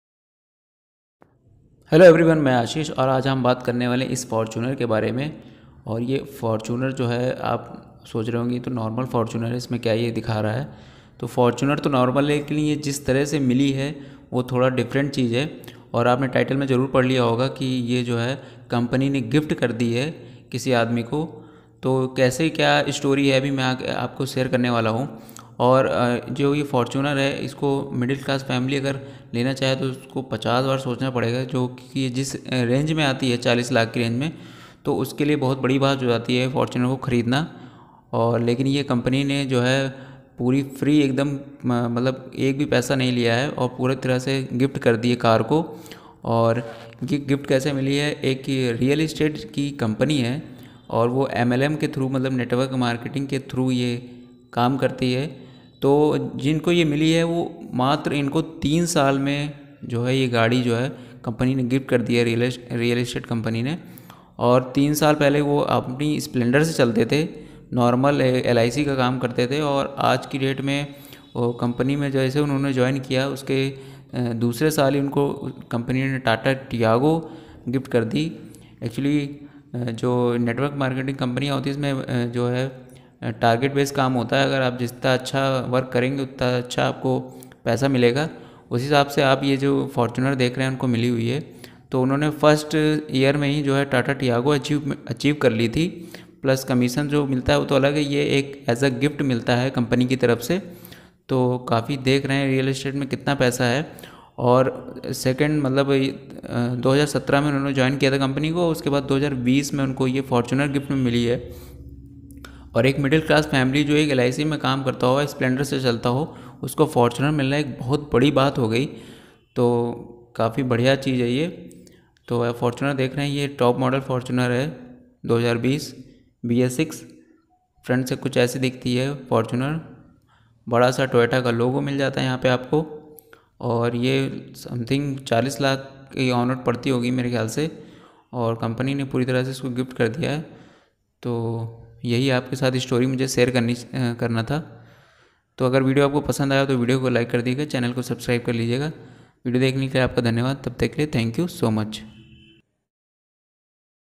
कंपनी ने गिफ्ट कर दी Toyota Fortuner || Toyota Fortuner bs6 2020 || Real and Exciting Story
Watch in this video:
Toyota Fortuner jisko lene ke lia middle class family ko 50 bar sochna padega, Lekin compony ne ye gadi gift kar di hai ek common man ko. Is video me vhi story share ki gai hai ki kis tarah ye gadi ek common man ko gift me mili hai.

About uploader:
Hey, I m Ashish from Lucknow, utter pradesh(UP 32). I recently completed my engineering.

Thanx for being here🙏...